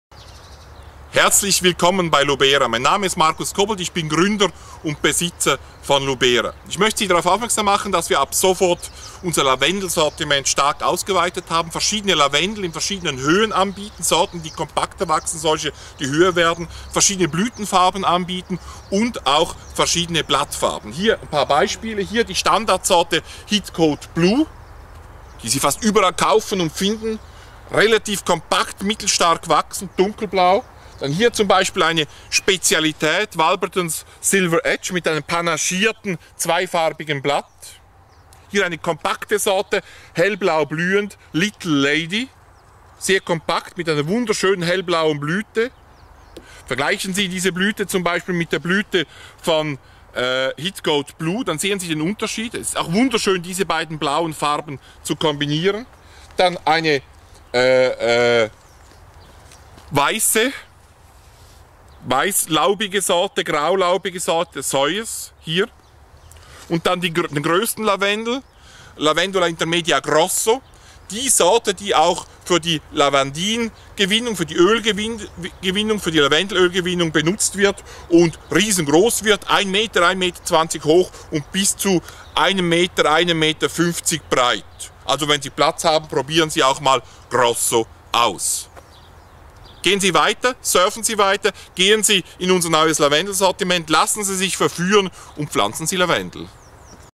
Markus Wochenshow 24.06.2011
Unser Startvideo für die Website vom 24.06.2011. Markus stellt Ihnen unser neues Lavendelsortiment vor.